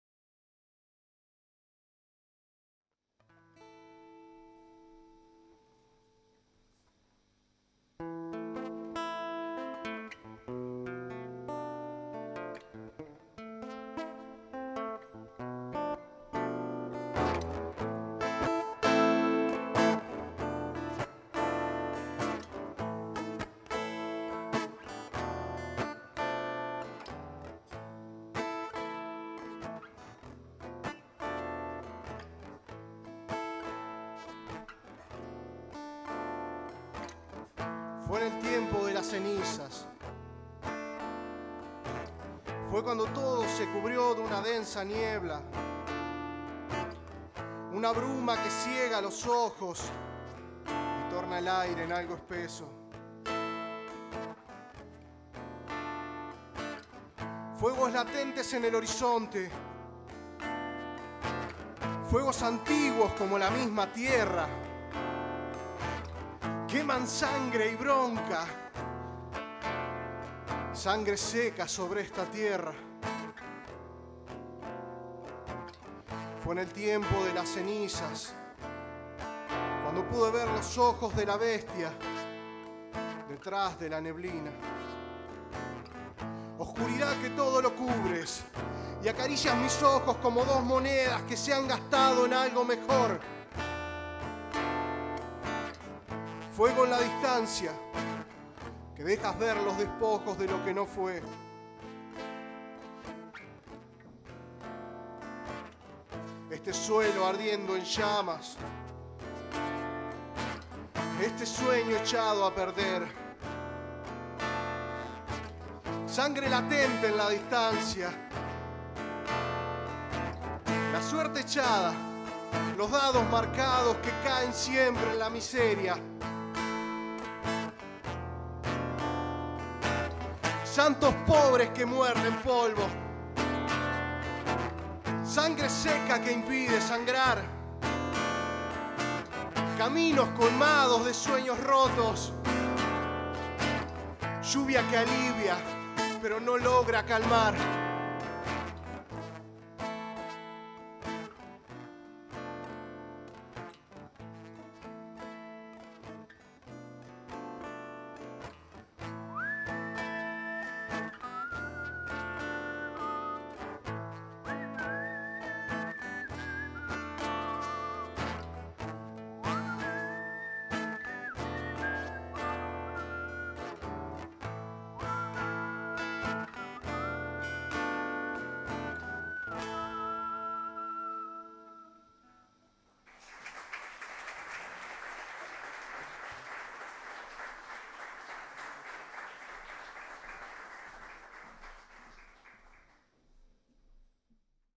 EL TIEMPO DE LAS CENIZAS - Versos para antes de...- de Germán Reiesuane
Presentación del Libro “Versos para antes de plantarme una flor como sombrero”  de Germán Reiesuane el 29 de Septiembre del año 2012.